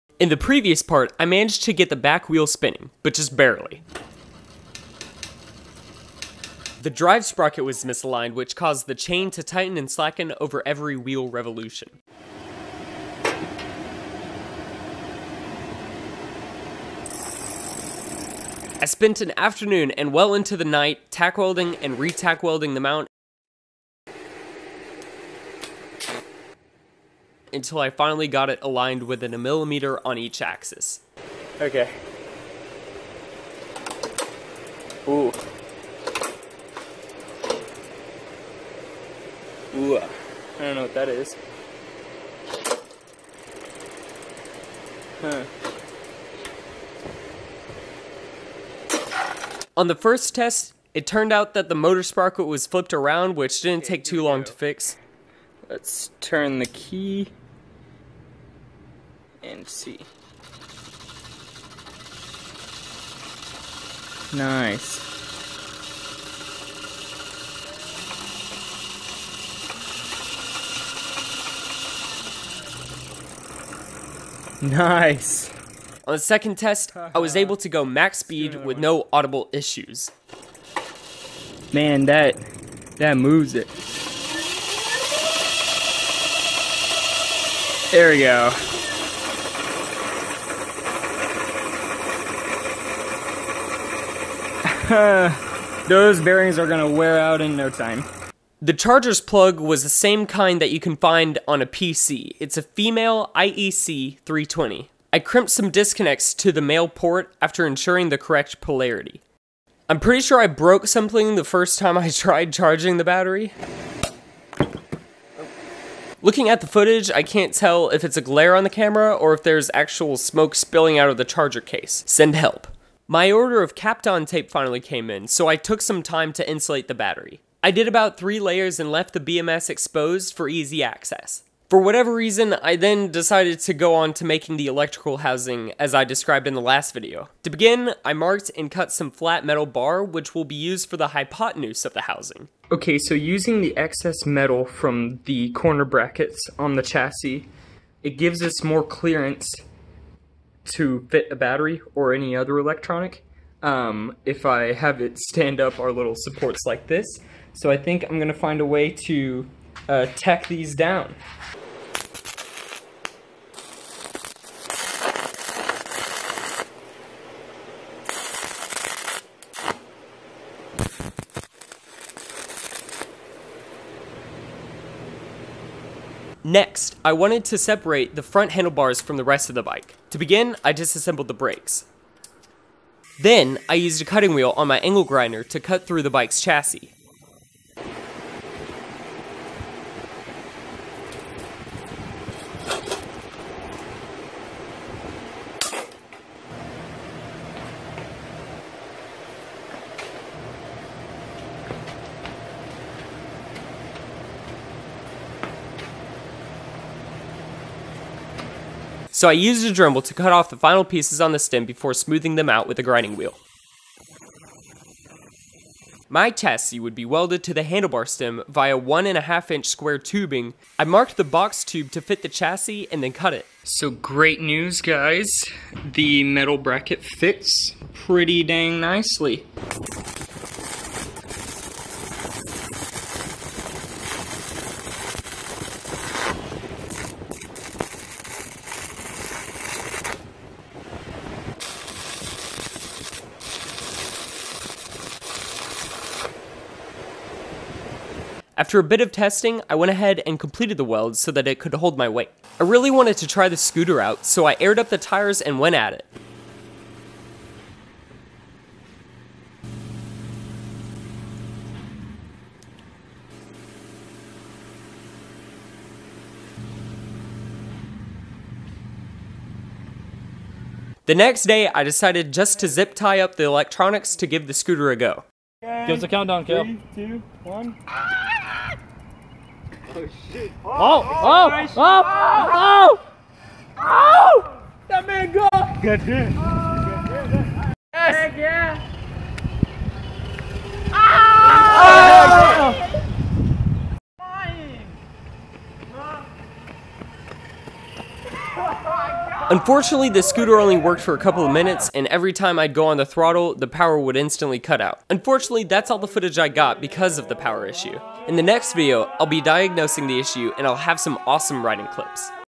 FIRST RIDE DIY Electric Scooter
FINALLY, everything has been put in place for the first test ride of my electric scooter from scratch. Sadly, there's some power issues I'll have to address next video, which will be coming very soon. I promise there will be more riding!

You can learn more about this DIY custom electric scooter project on my This article includes the 1000W motor we used, along with the 18650 batteries, motor speed controller, throttle, battery management system, welder, and angle grinder.